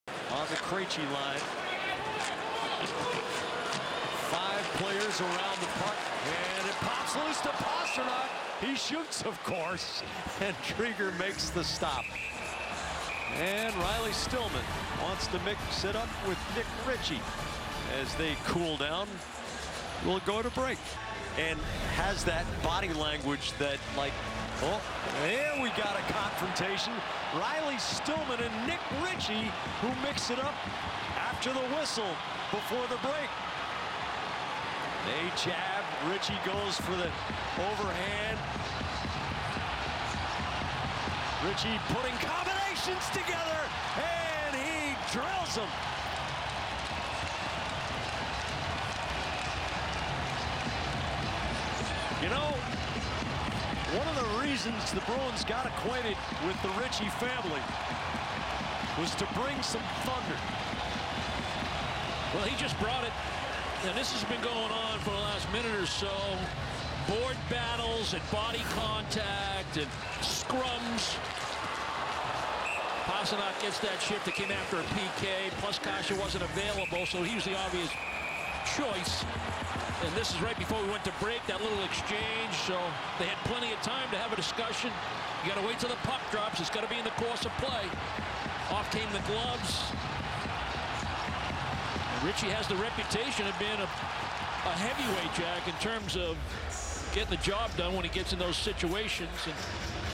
Nick Ritchie fights Riley Stillman 3/5/20
Nick Ritchie fights Riley Stillman, on March 5, 2020.